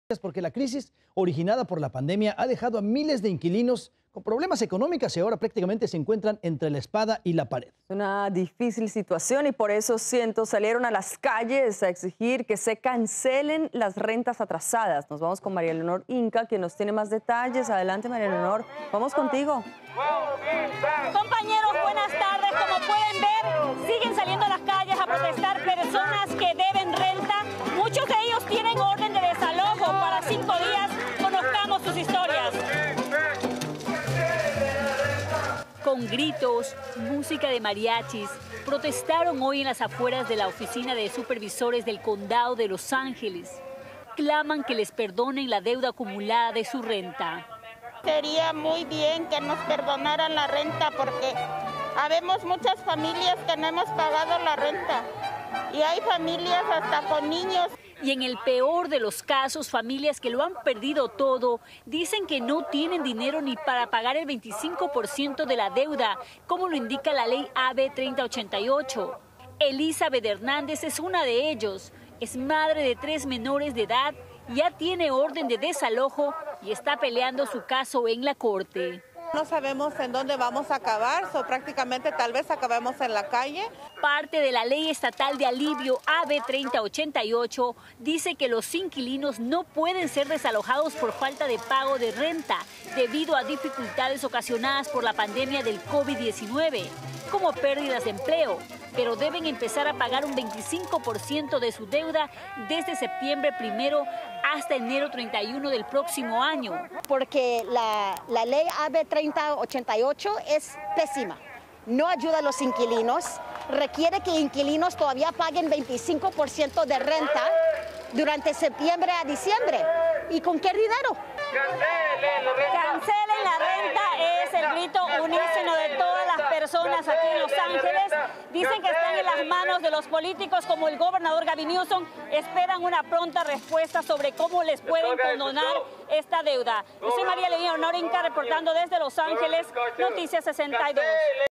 INQUILINOS SALEN A LAS CALLES A EXIGIR QUE CANCELEN LAS RENTAS ATRASADAS – Noticias 62
La crisis originada por la pandemia ha dejado a miles de inquilinos con problemas económicos y ahora están entre la espada y la pared, por eso cientos salieron a las calles a exigir que se cancelen las rentas atrasadas.
María Leonor Inca, quien nos tiene más detalles.